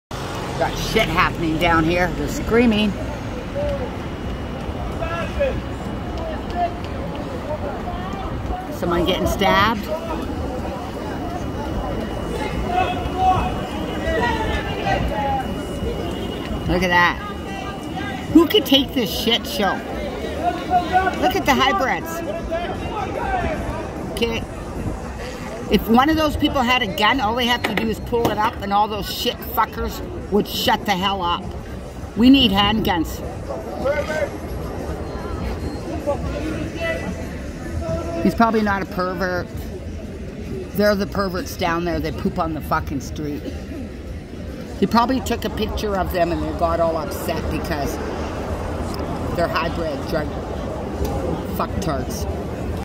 Hybrid Druggies Screaming Pervert 😱 When They're Famous 4 Tent Rape DTES Vancouver 😡😠🤬
At Oppenheimer park a woman was complaining about being raped in the tent and some other woman named  I believe Sarah Blige said that that's part of being I've been in the tenting community and not to complain about freaking rate can you believe it this person stands up for these f*** tarts and in a new world government they're all going to be slaughtered